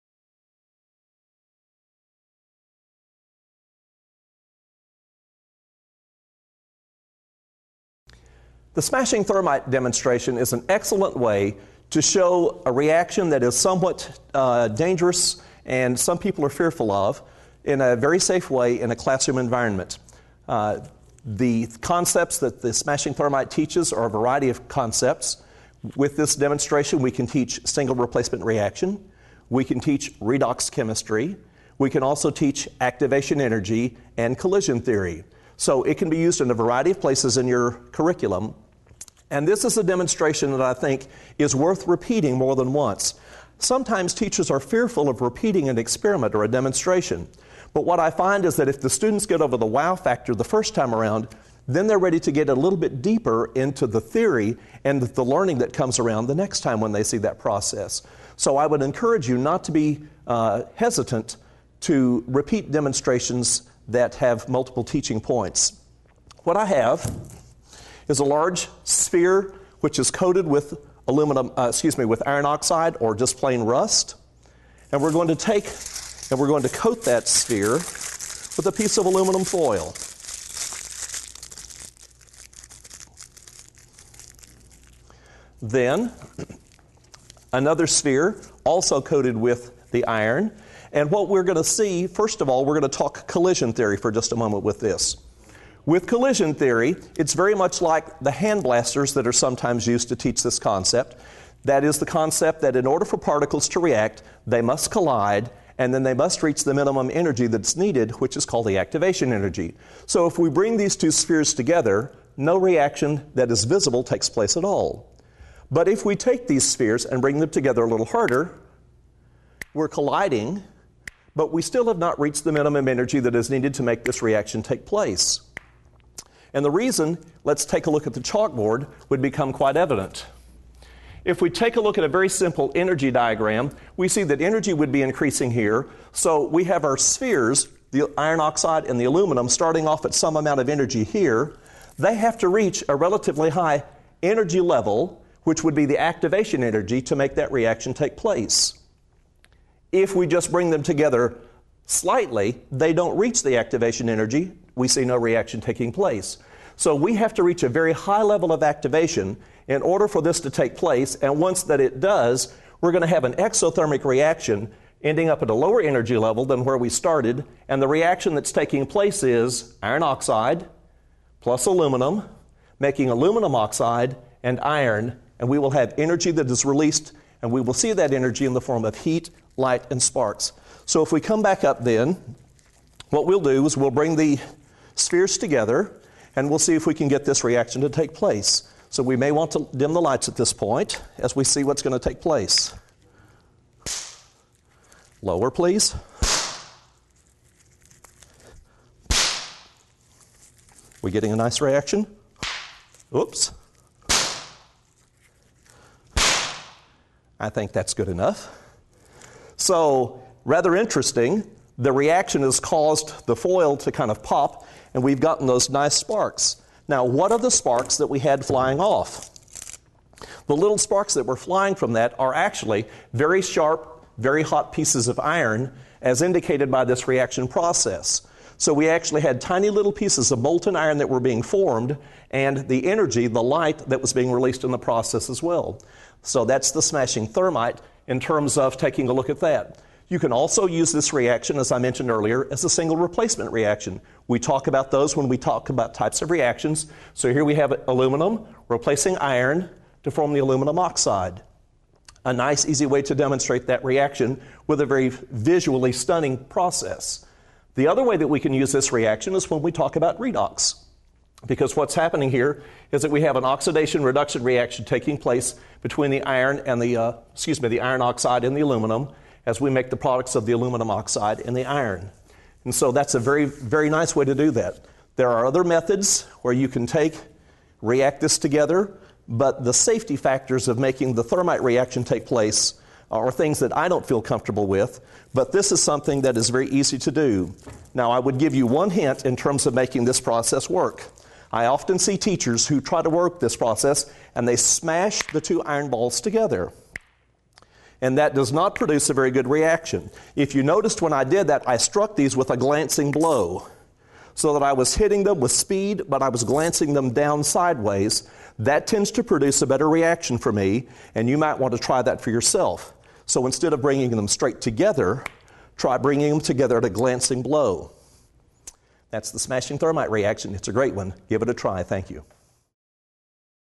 Smashing Thermit Reaction
Take two rusty iron balls, wrap one in aluminum foil, and bang them together—then watch the sparks fly!